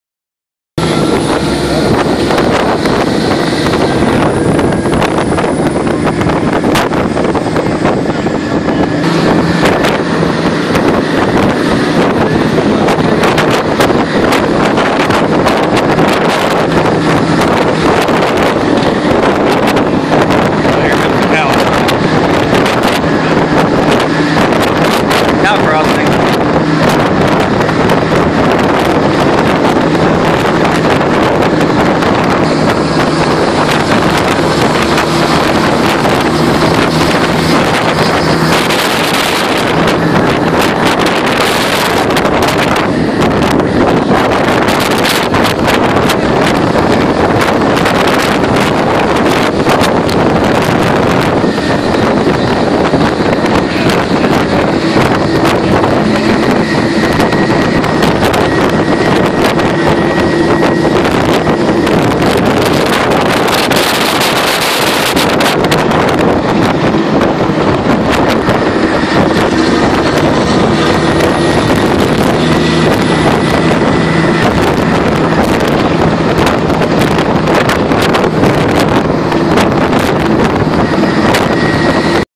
Rolling in the Cattle Truck during AT
Army training has never been so much fun.